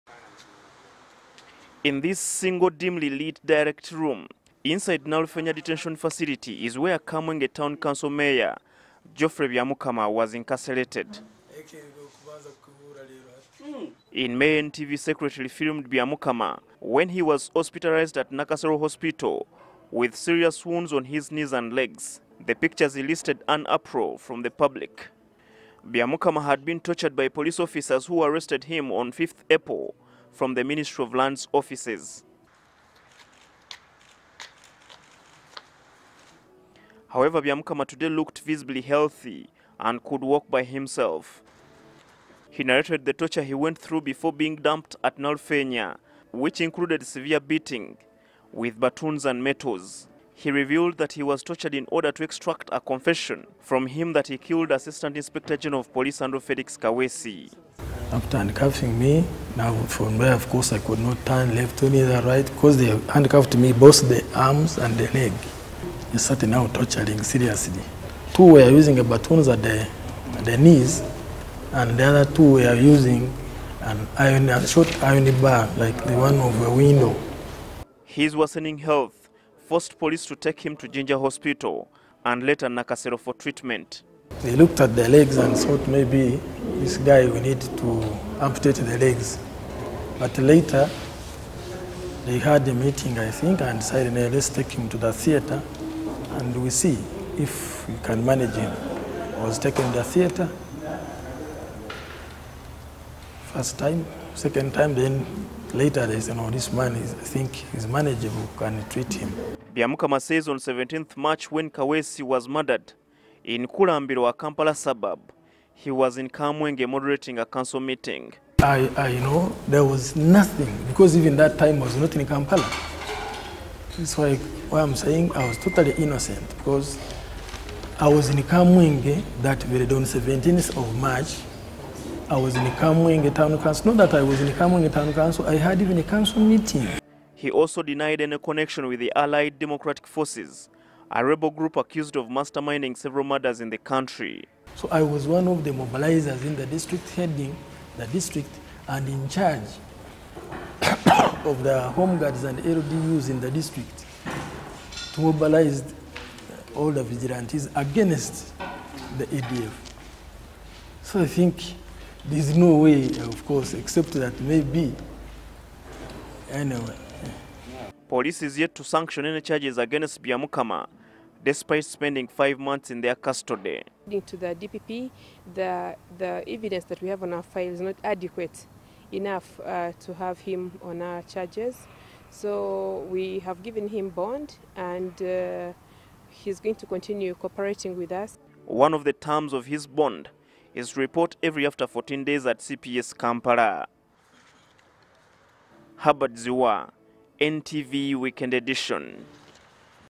Police releases Kamwenge mayor Geoffrey Nyamukama on bond and files no charges against him
Police have released on bond the Kamwenge mayor Geoffrey Byamukama who was arrested, severely tortured and detained incommunicado in connection with the murder of Assistant Inspector General of Police Andrew Felix Kaweesi, his driver and body guard.
Despite his release, police is yet to file any charges against him five months after his arrest.